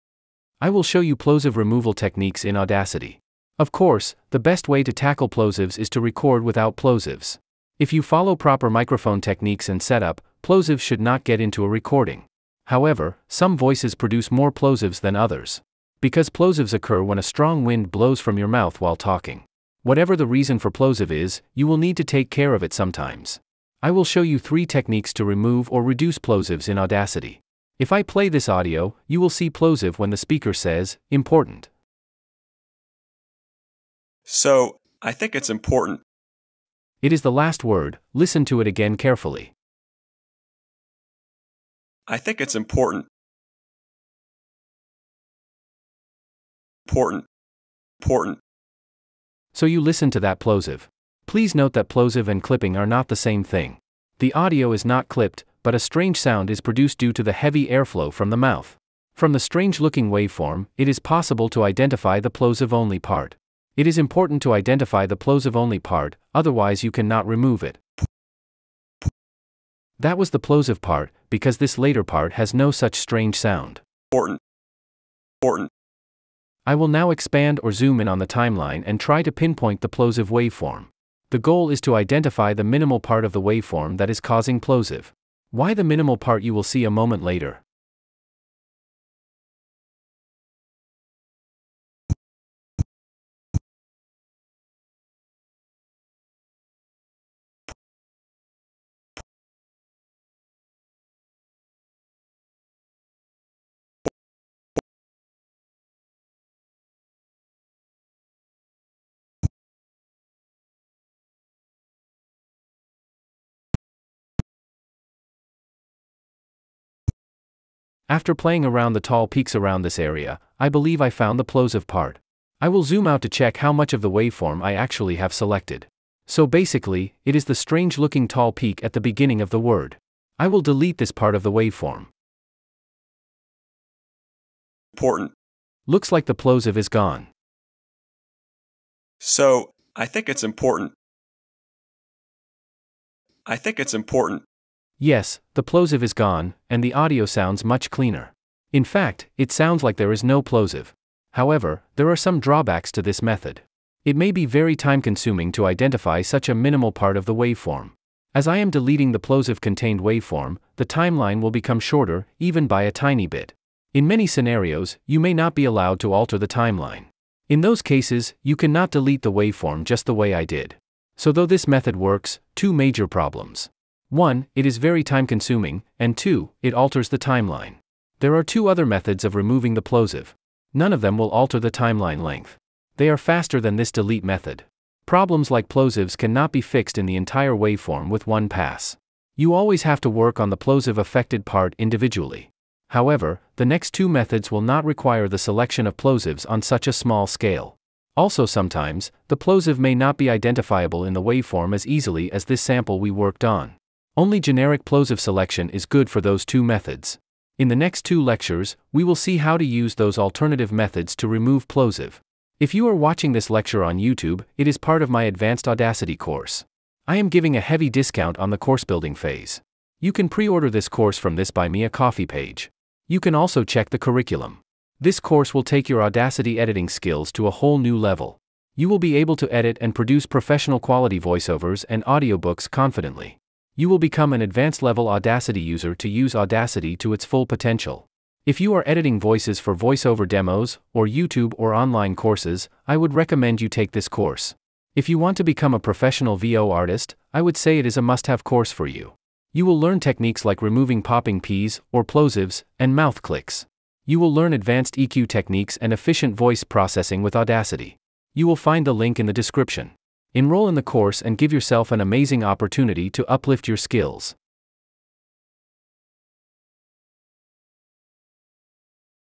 How to Remove Plosives in Audacity
How to Remove Plosives or popping P's in voice with Audacity.

Get the Free Sound Better PDF guide and 12 must-follow checkpoints to achieve for Pro Voice-Over PDF